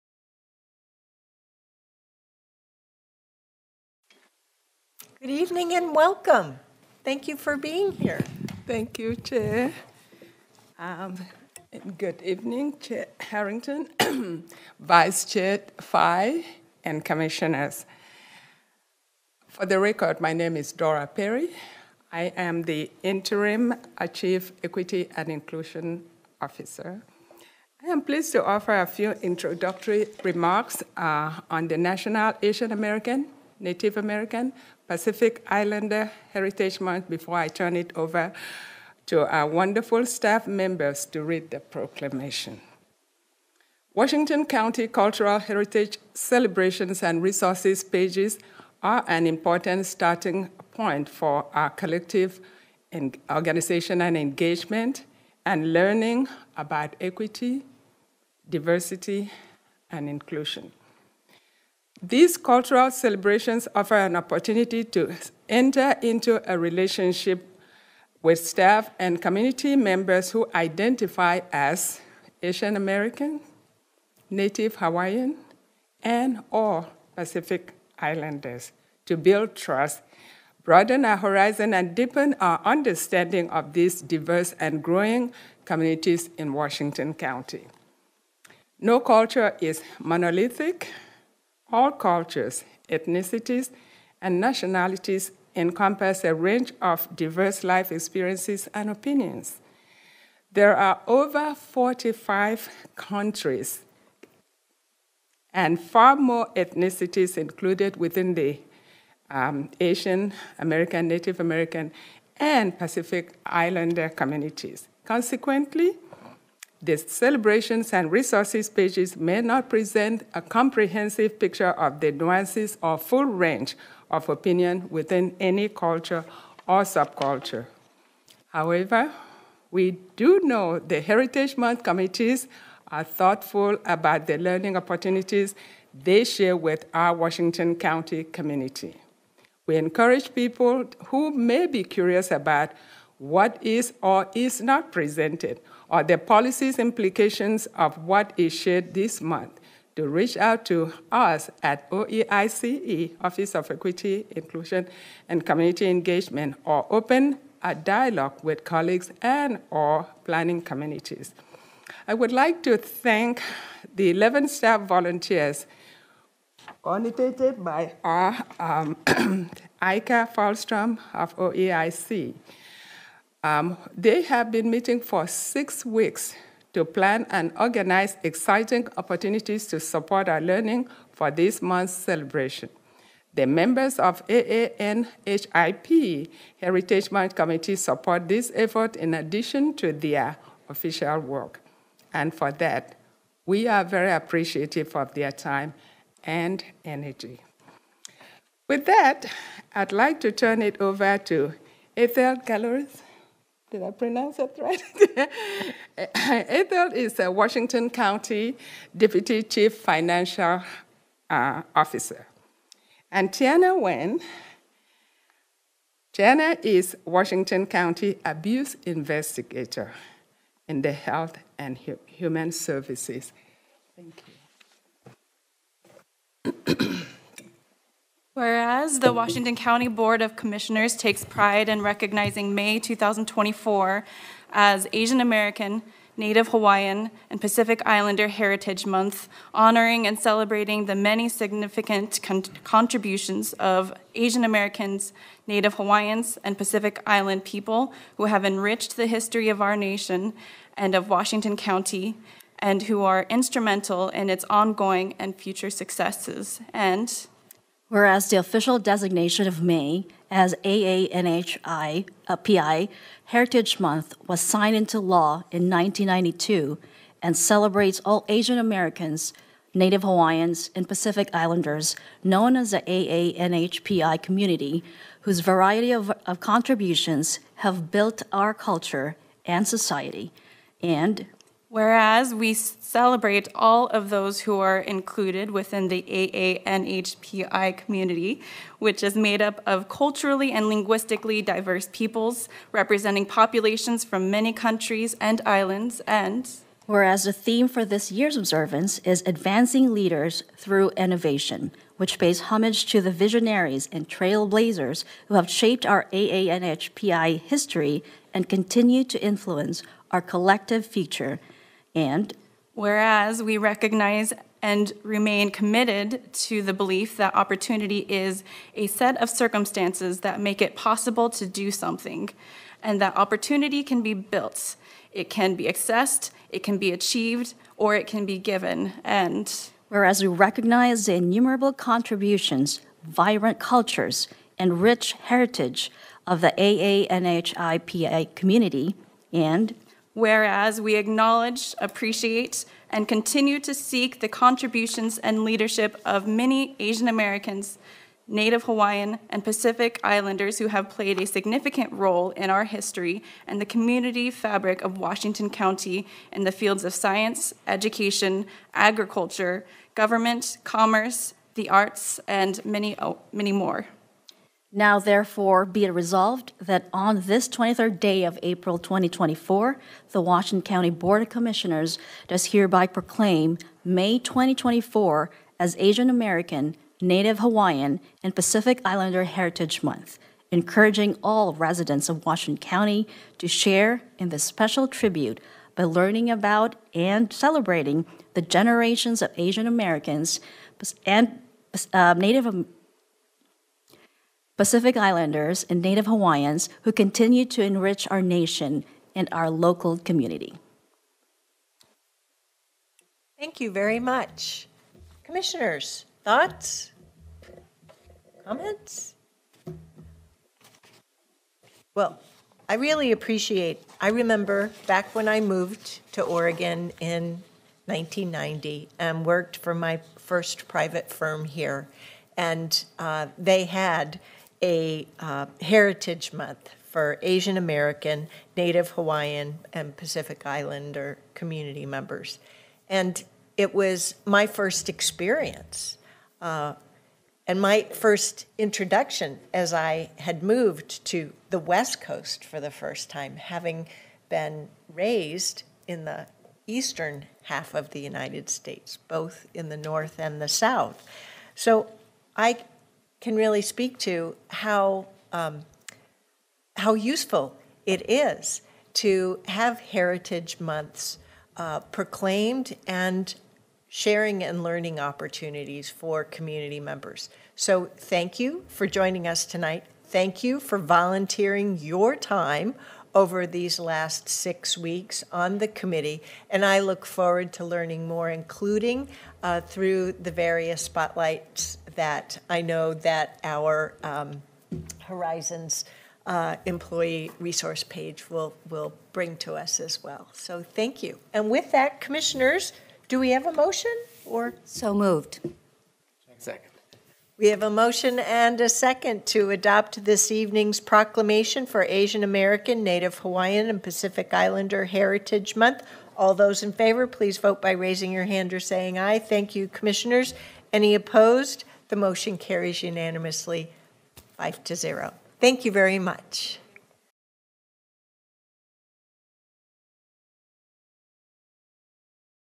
Asian American Native Hawaiian Pacific Islander Heritage Month Proclamation
Asian American Native Hawaiian Pacific Islander Heritage Month Proclamation - Segment pulled from Washington County Board of Commissioners - Public Meeting 04/23/24.